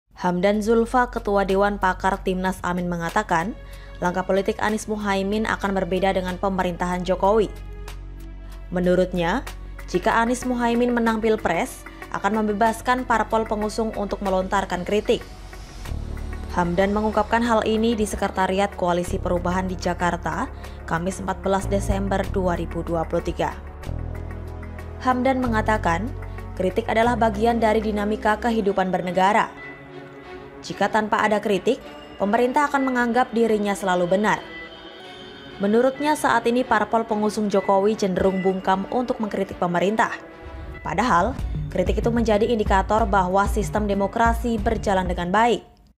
Janji Kubu Anies-Cak Imin, Tak Akan seperti Pemerintahan Jokowi
Ketua Dewan Pakar Tim Nasional (Timnas) Pemenangan Anies-Muhaimin (Amin), Hamdan Zoelva mengatakan, langkah politik Anies-Muhaimin akan berbeda dengan pemerintahan Presiden Joko Widodo.
Ia mengungkapkan, jika memenangkan Pemilihan Presiden (Pilpres) 2024, Anies dan Muhaimin akan membebaskan partai politik (parpol) pengusung untuk memberikan kritik.
Video Jurnalis: HAM, MIC
Penulis : Tentang Guritno
Penulis Naskah: Dariz Kartika
Narator: Dariz Kartika
Video Editor: Dariz Kartika
Produser: Dandy Bayu Bramasta